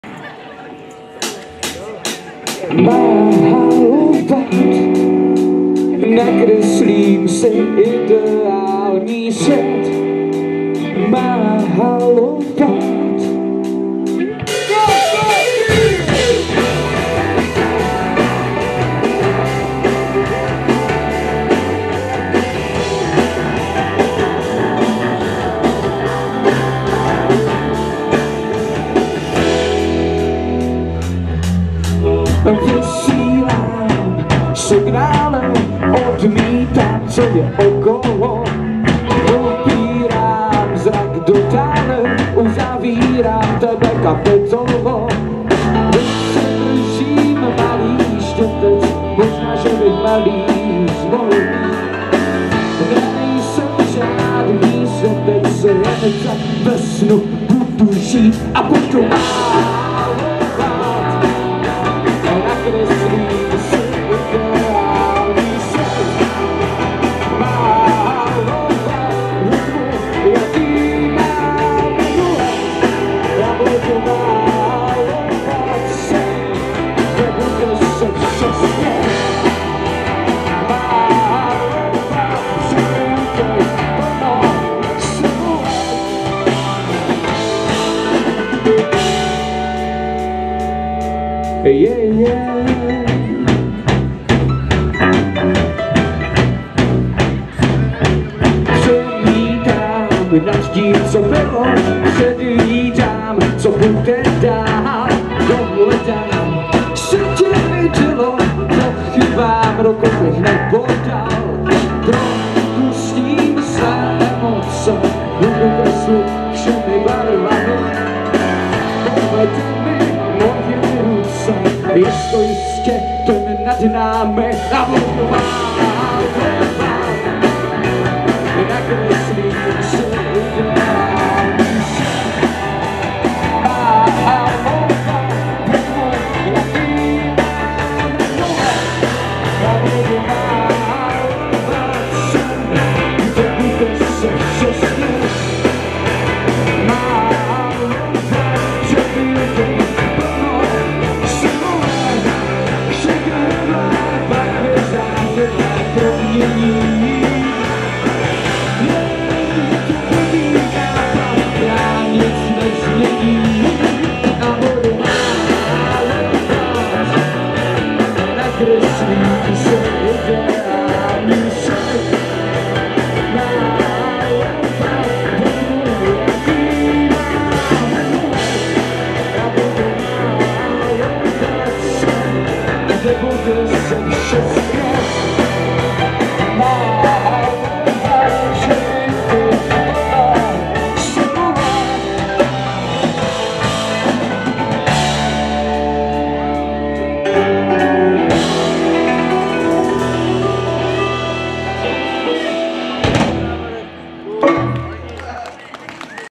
KYJOV-Skvělá kapela ŠROTI z Dolních Bojanovic na Masarykově náměstí
- u příležitosti  akce-30.let místní Sjednocené organizace nevidomých a slabozrakých
- 24. září 2021, 20.00 hodin, Masarykovo náměstí v Kyjově